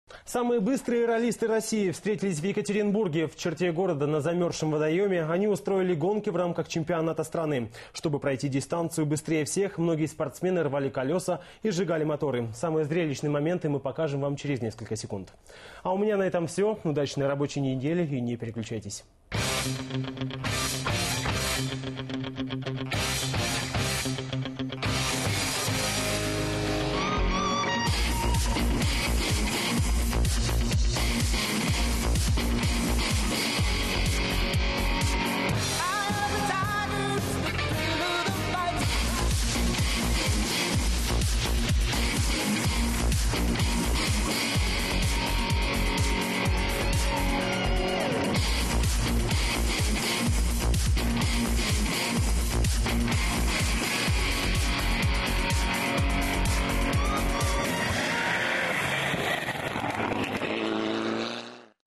Ралли на льду. Новости. Екатеринбург
Самые быстрые раллисты России встретились в Екатеринбурге. В черте города на замерзшем водоеме они устроили гонки в рамках Чемпионата страны. Чтобы пройти дистанцию быстрее всех, многие спортсмены рвали колеса и сжигали моторы.